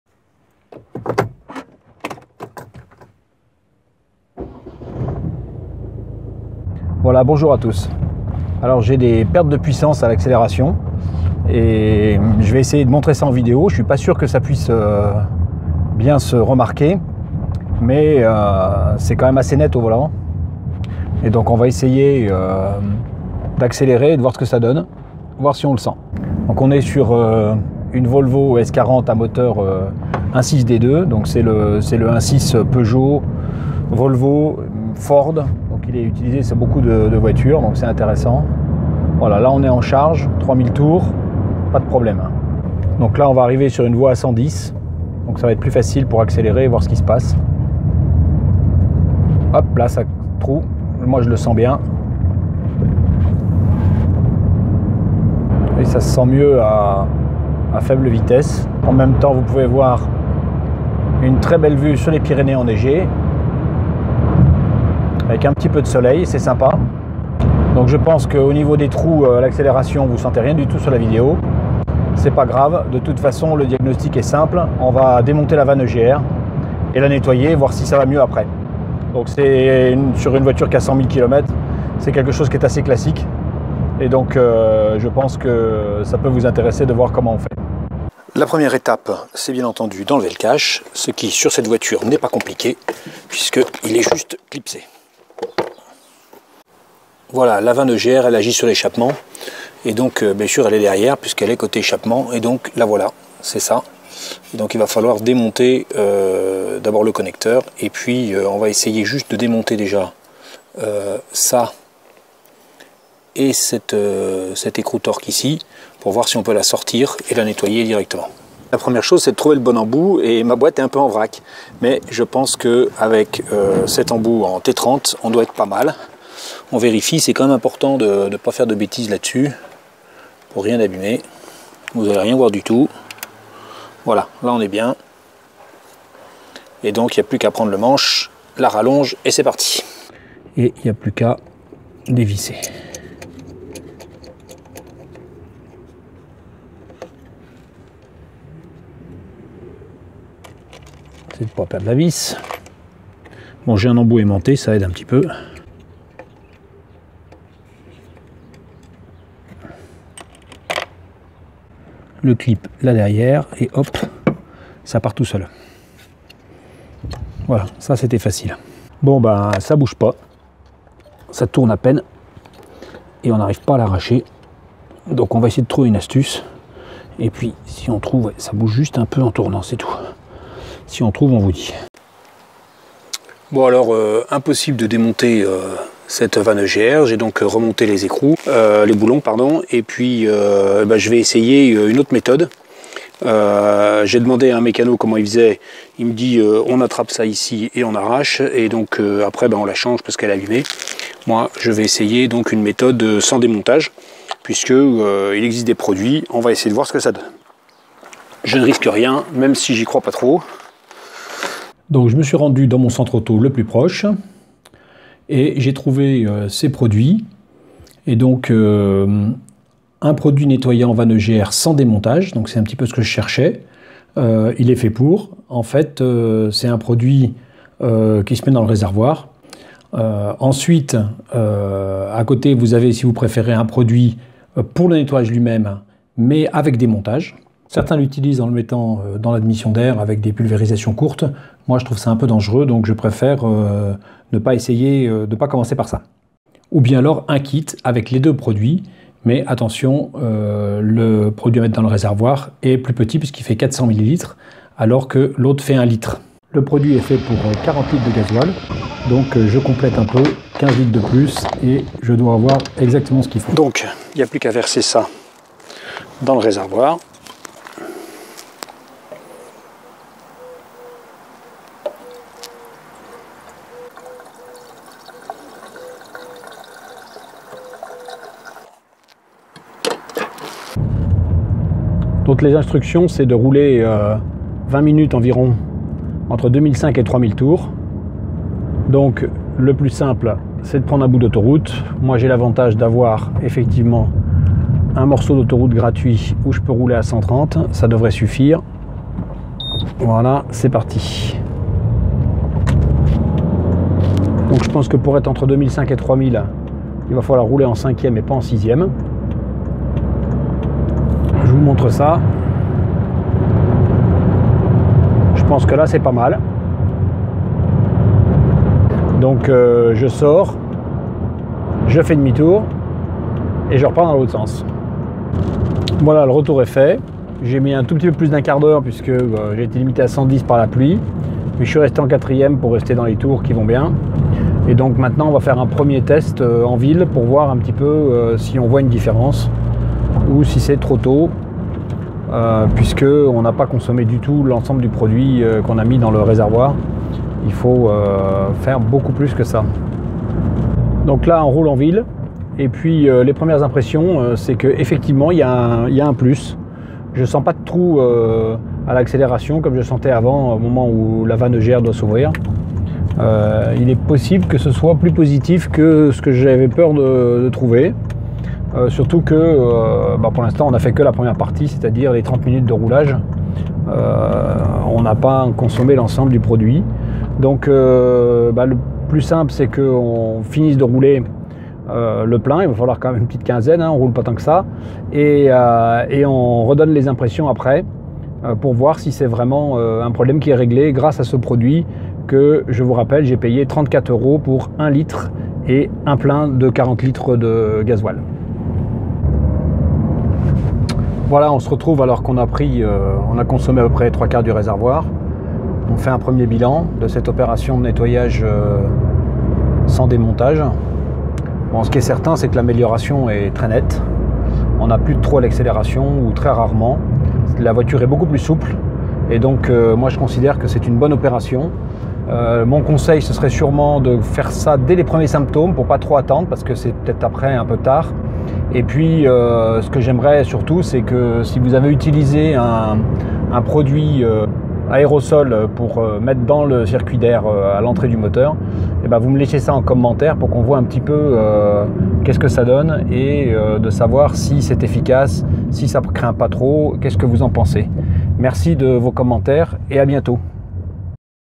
Nettoyage vanne EGR sans démontage avec un produit BARDHAL
Nettoyage de la vanne EGR. Comme je n'arrive pas à la démonter, je test un produit BARDAHL  de nettoyage vanne EGR à mettre dans le réservoir.
Si vous avez testé un autre produit, décrivez votre expérience dans les commentaires. Merci

Une nouvelle vidéo de Piston et Savon !